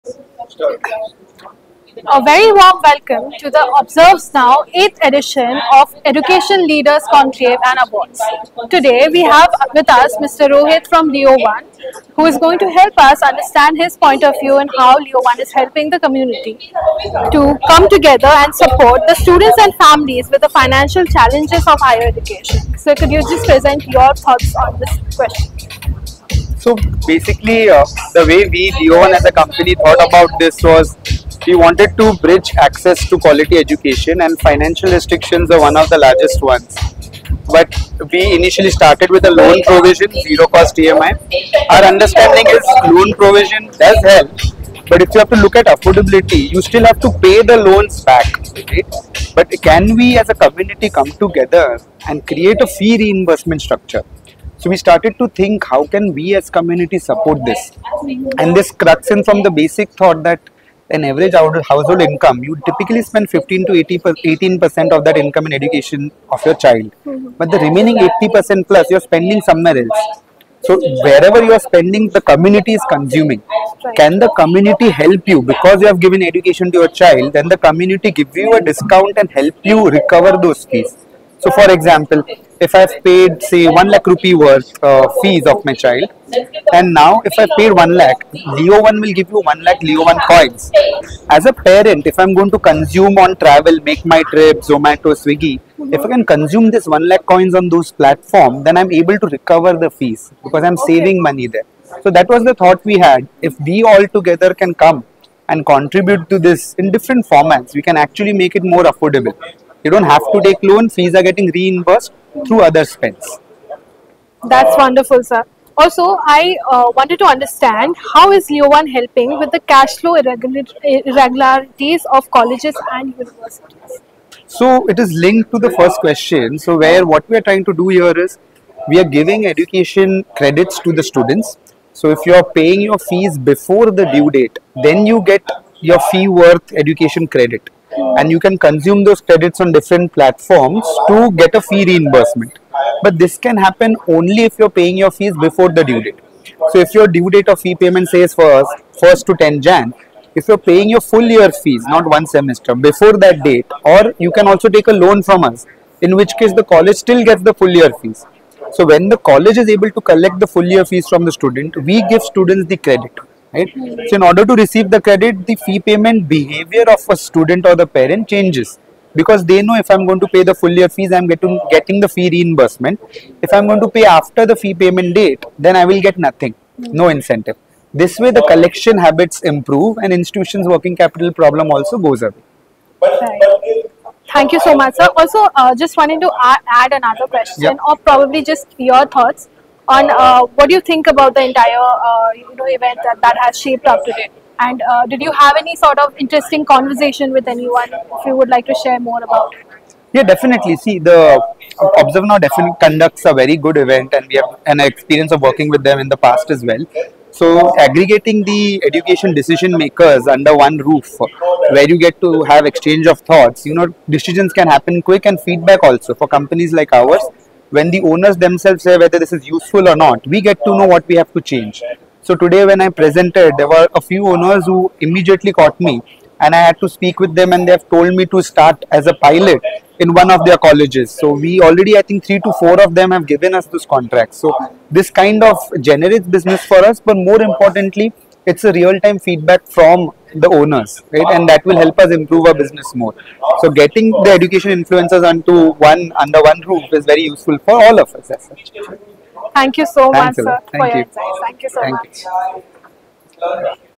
Interview with Rohit Gajbhiye, CEO & Founder of LEO1 at the 8th
Interview with Rohit Gajbhiye, CEO & Founder of LEO1 at the 8th Edition of the Education Leaders Conclave and Awards in Mumbai! This event, presented by NxtWave and powered by LEO1.